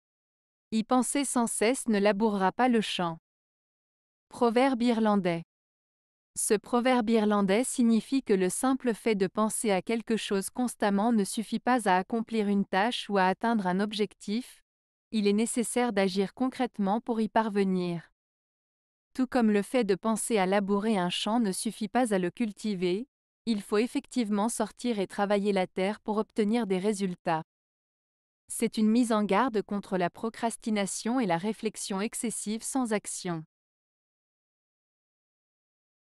Y penser sans cesse ne labourera pas le champ. - Proverbe Irlandais
Ce proverbe irlandais signifie que le simple fait de penser à quelque chose constamment ne suffit pas à accomplir une tâche ou à atteindre un objectif, il est nécessaire d'agir concrètement pour y parvenir. Tout comme le fait de penser à labourer un champ ne suffit pas à le cultiver, il faut effectivement sortir et travailler la terre pour obtenir des résultats. C’est une mise en garde contre la procrastination et la réflexion excessive sans action.